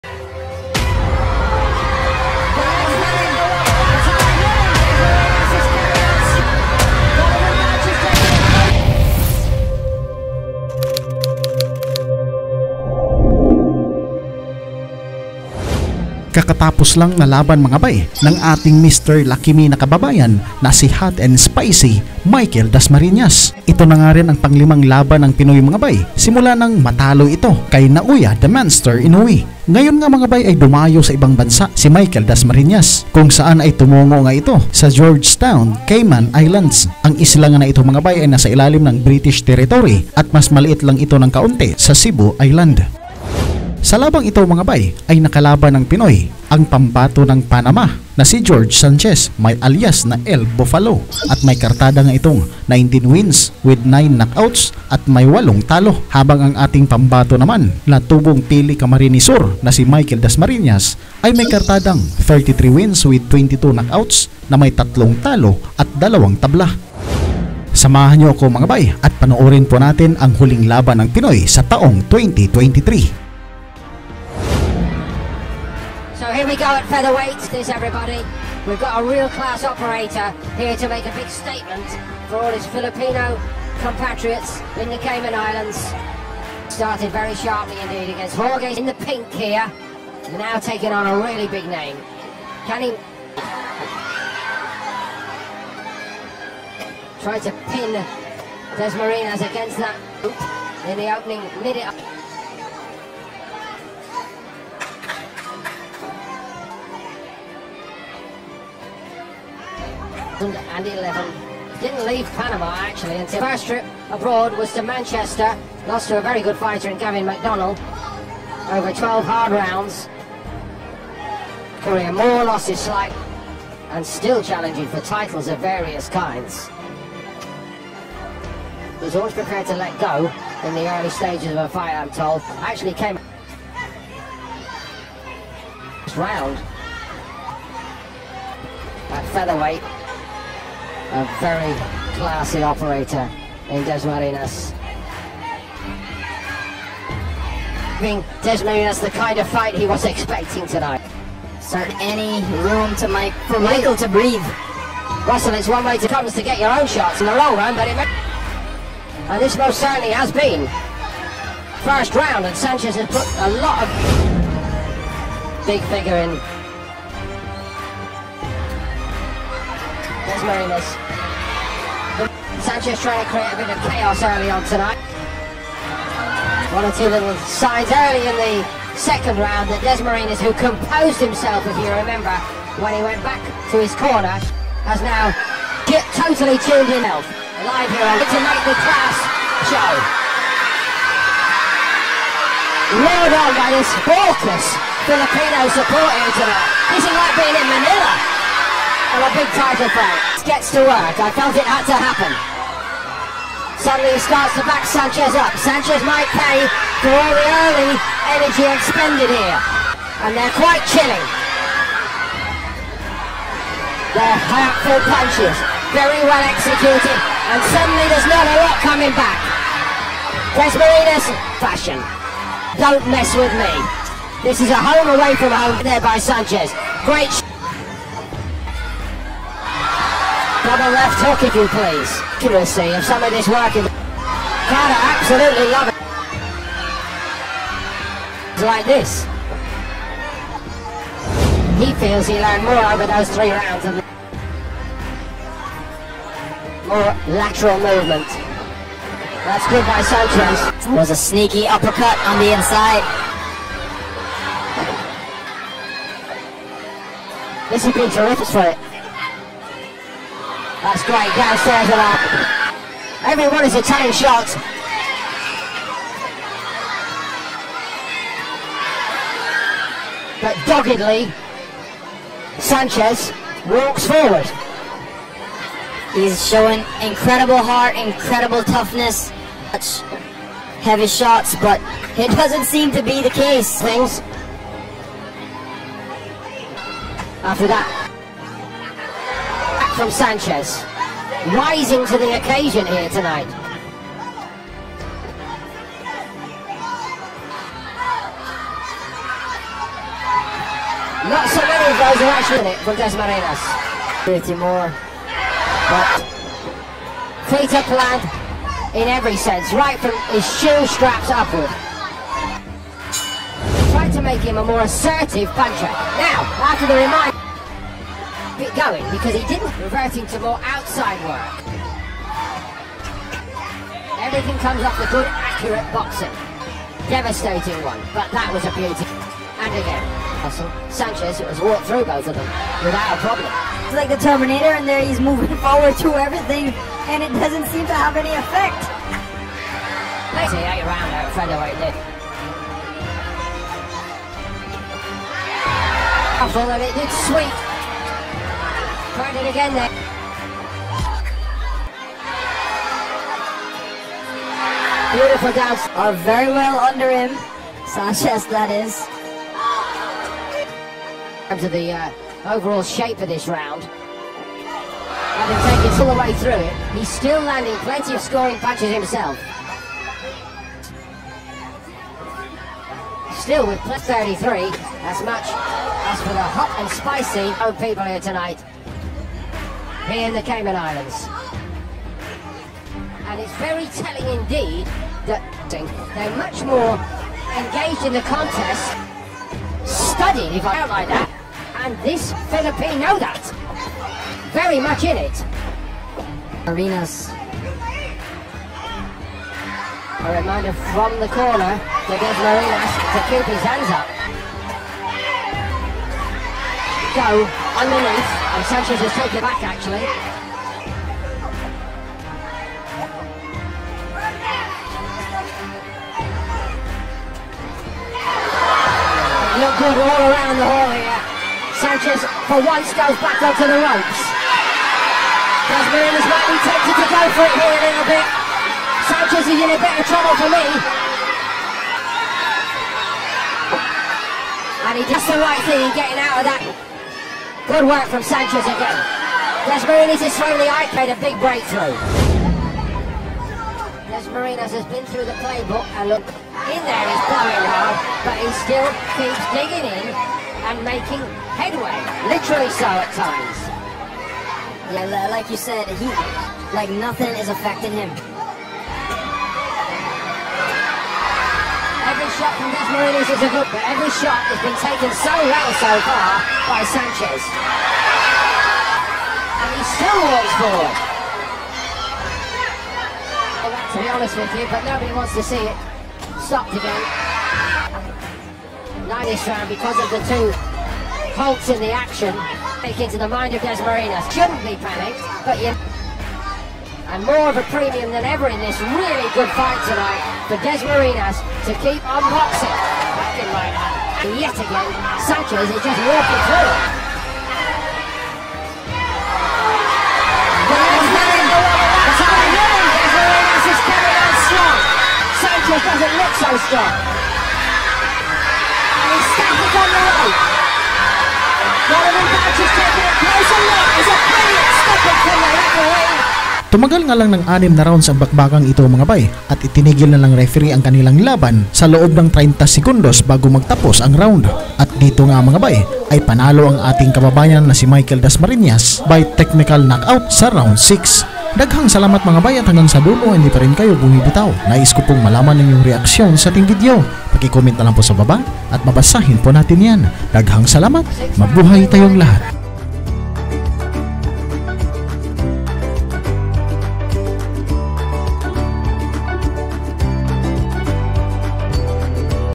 December 9, 2023 | MALA-TERMINATOR NA KALABAN Hindi Kinaya ang Hot 'N Spicy PINOY POWER!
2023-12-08  |  Lions Centre, George Town, Cayman Islands
Michael Dasmarinas vs Jorge Sanchez
Jorge Sanchez vs Michael Dasmarinas
Dasmarinas vs Sanchez
Sanchez vs Dasmarinas
Hot 'N Spicy
El Bufalo

Pasuporta sa ating 2nd Channel mga Bai: HighlightsPH WORLD